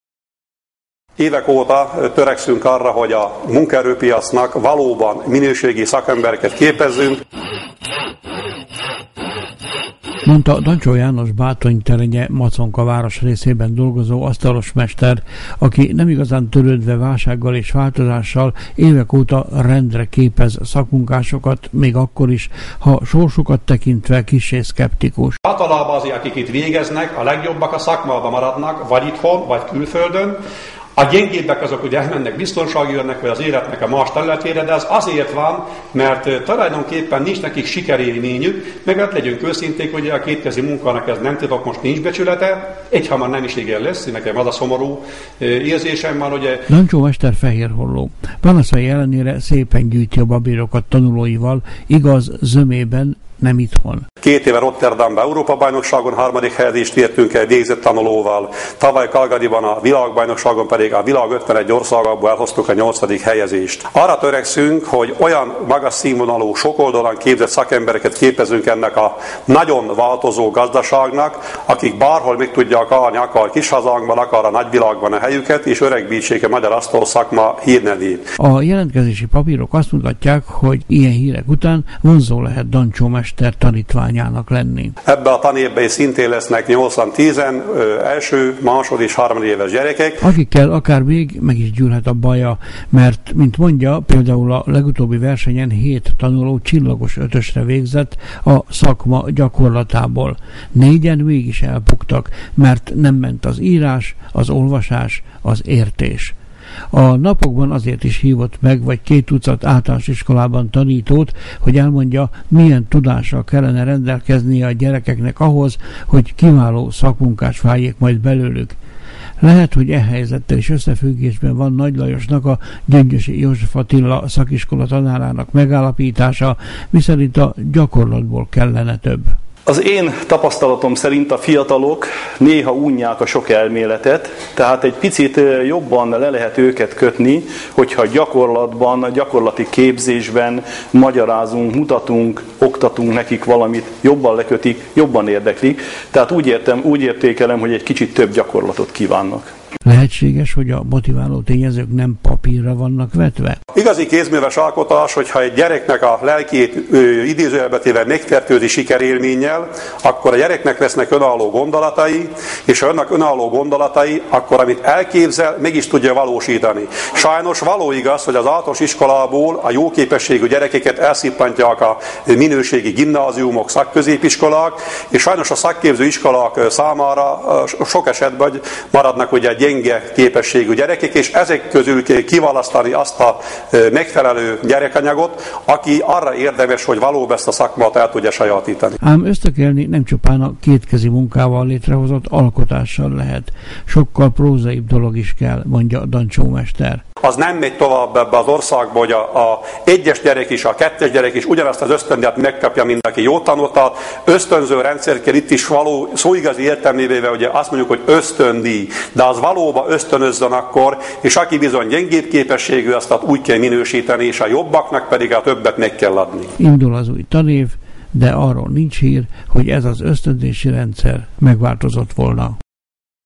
Szakmunkást, de hogyan?
Dancsó János maconkai asztalos mester műhelyében készült Zengő Árpád videoriportja.